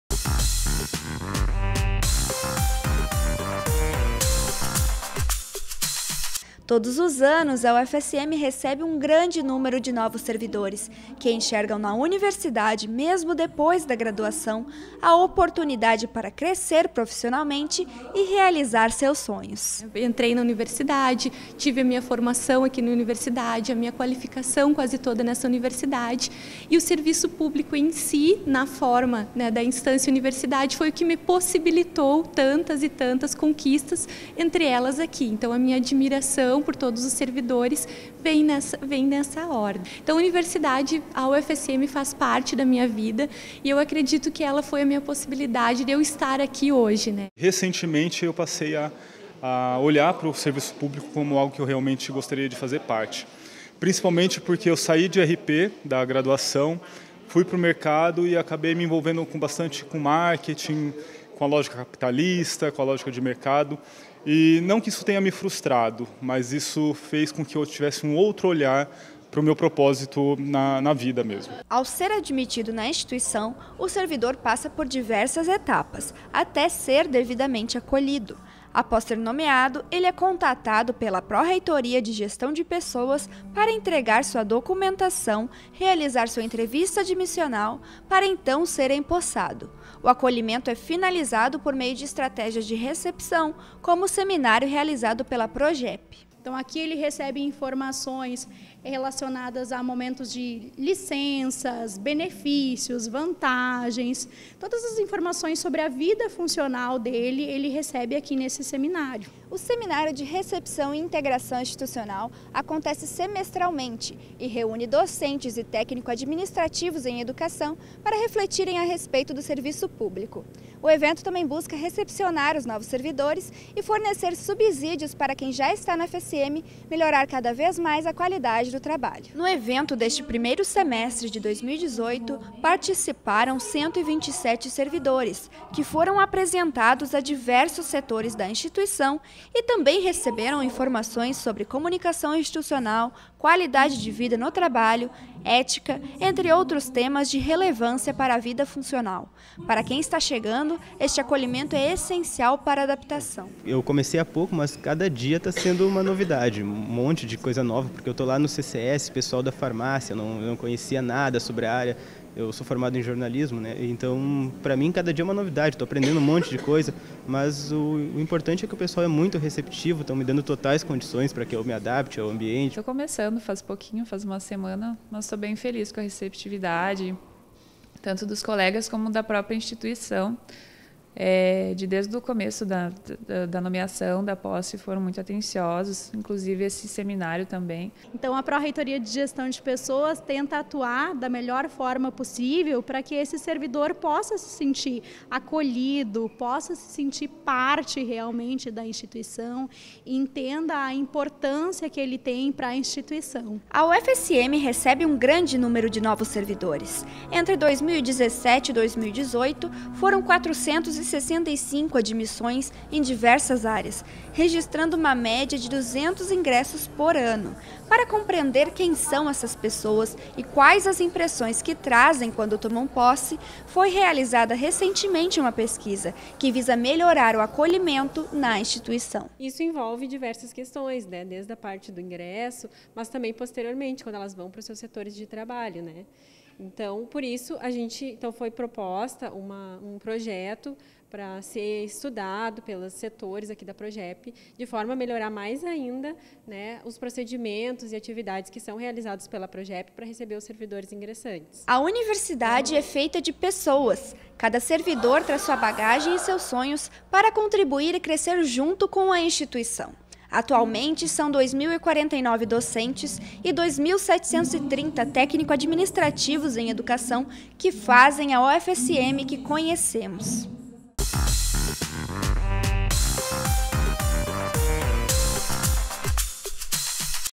Recepção aos novos servidores - Acontece na UFSM
A Pró-Reitoria de Gestão de Pessoas (PROGEP) recepcionou novos servidores técnico-administrativos em educação em uma semana de palestras e apresentação da estrutura Universidade Federal de Santa Maria.